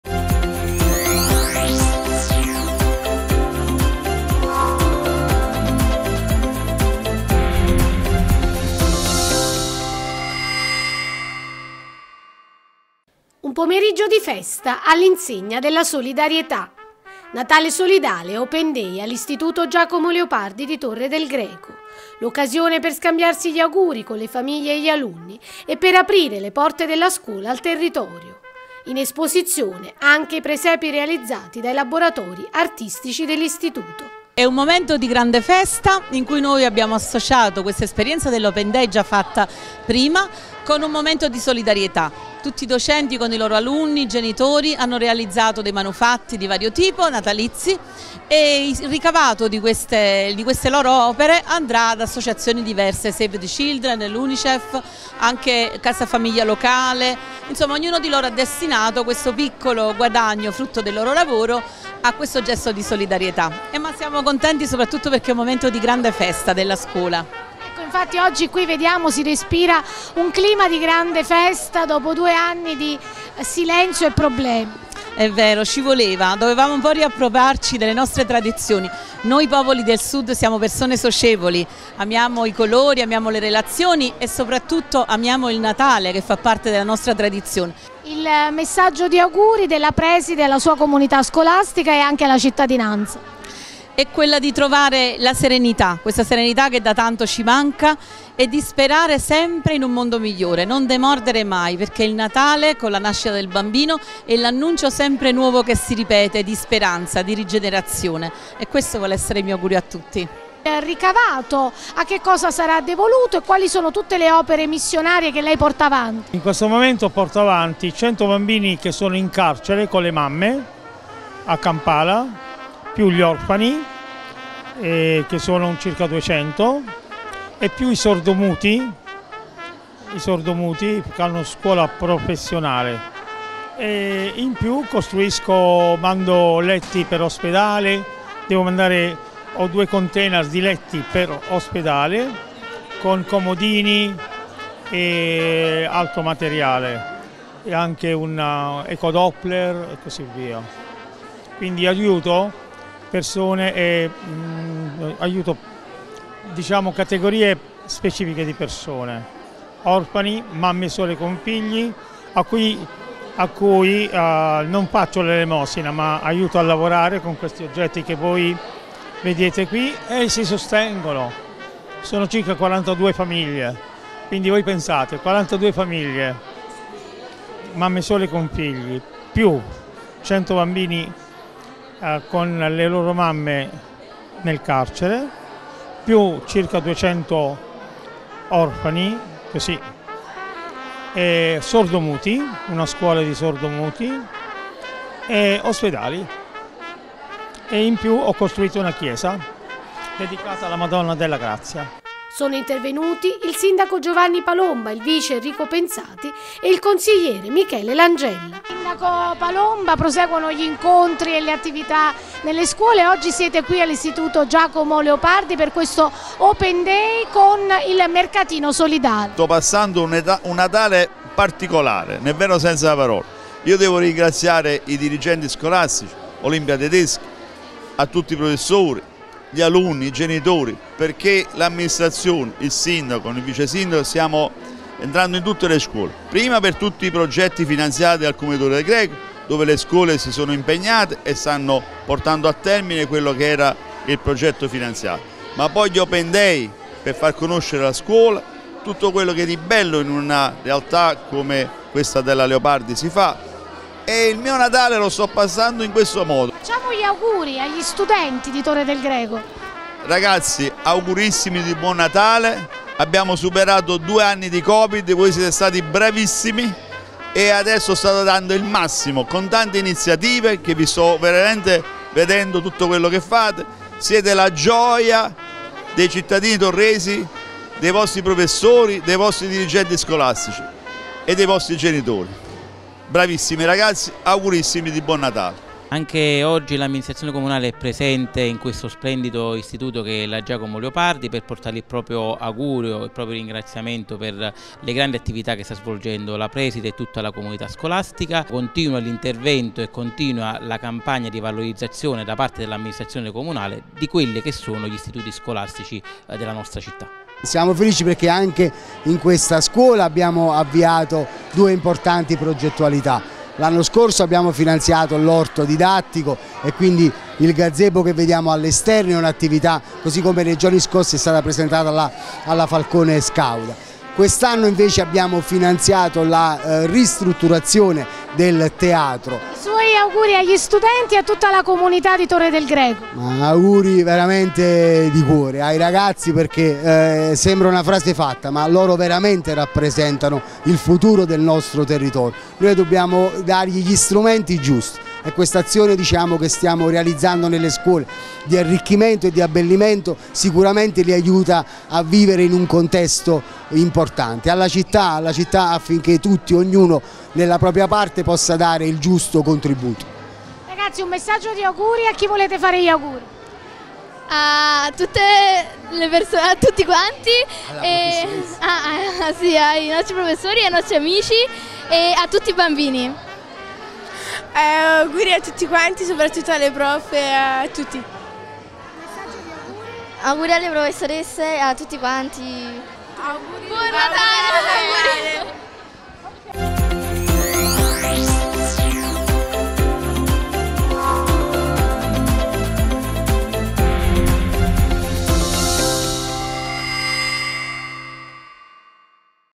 Natale solidale Open day all'Istituto Giacomo Leopardi
Un pomeriggio di festa all'insegna della solidarietà.

Natale solidale e Open day all'istituto Giacomo Leopardi di Torre del Greco.

L'occasione per scambiarsi gli auguri con le famiglie e gli alunni e per aprire le porte della scuola al territorio.

In esposizione anche i presepi realizzati dai laboratori artistici dell'istituto.

Sono intervenuti il sindaco Giovanni Palomba, il vice Enrico Pensati e il consigliere Michele Langella.